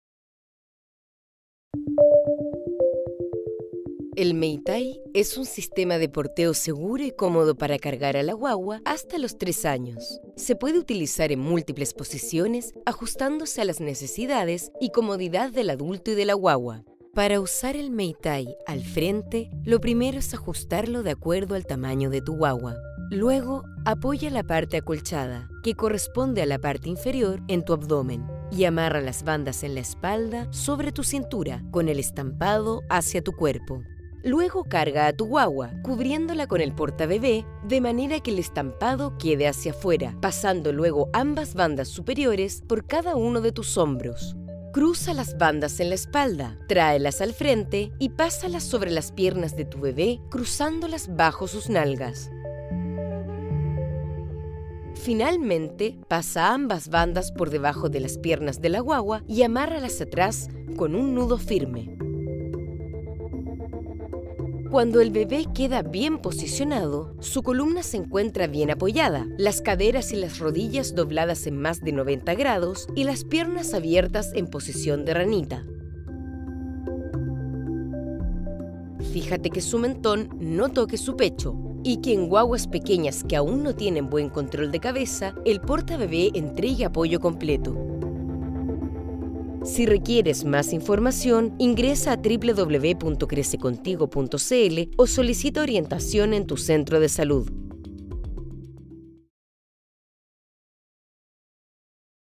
Técnica de Amarre de portabebé tipo Mei Tai Delante _ Chile Crece Contigo
Instrucciones de amarre del portabebé tipo Mei tai con la guagua al frente.
Portabebé entregado por el Programa de Apoyo al Recién Nacido del Sistema Chile Crece Contigo